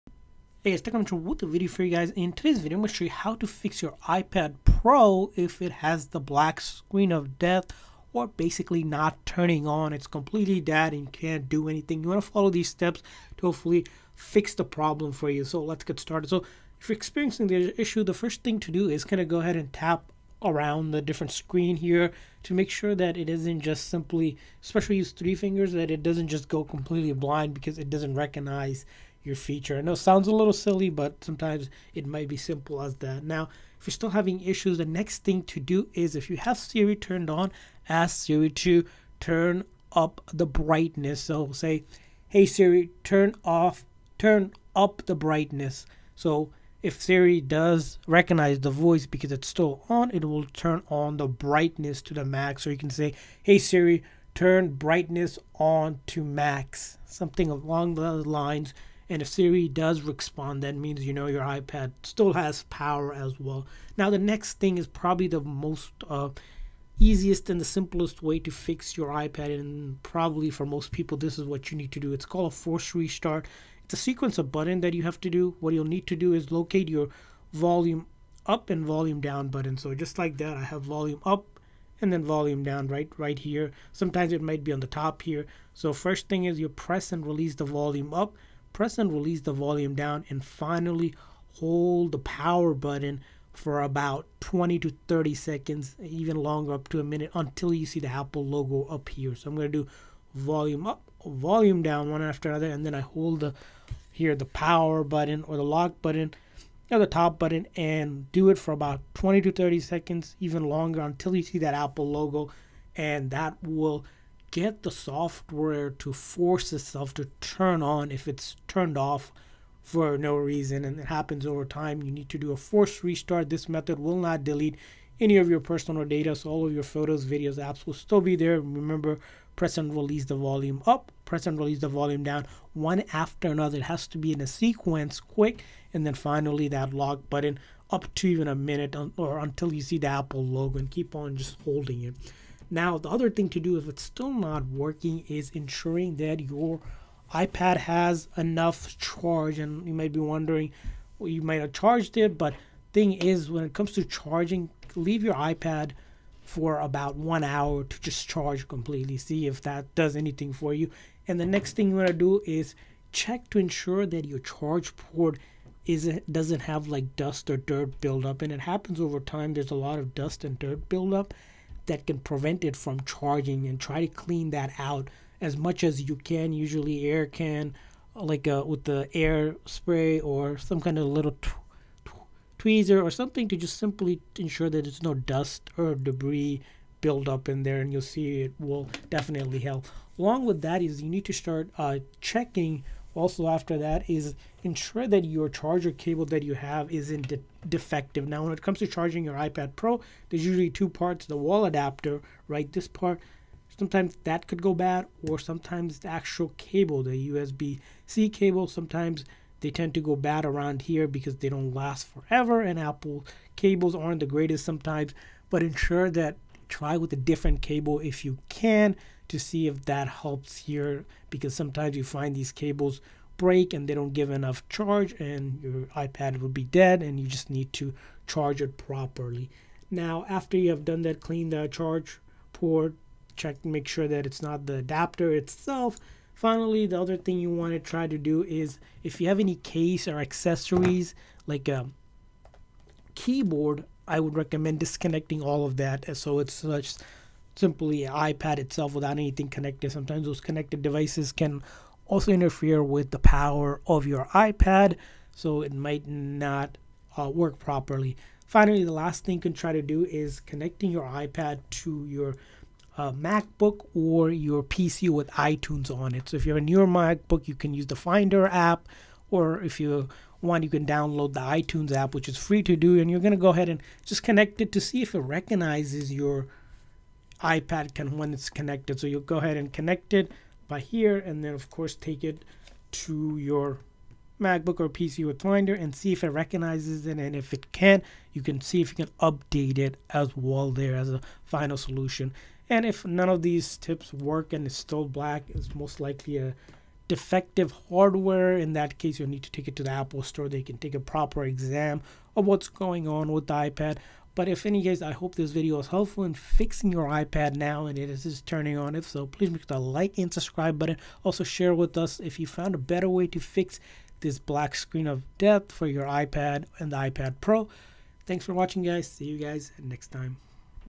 iPad Pro Black Screen of Death (FIXED)
In this video we show you how to fix your Apple iPad Pro Black Screen of Death issue. If your iPad Pro is not working or does not show anything on the screen beside the black color, here's all the troubleshooting steps you can do to fix this problem. These simple step by step can be used to fix any model iPad and even iPhone!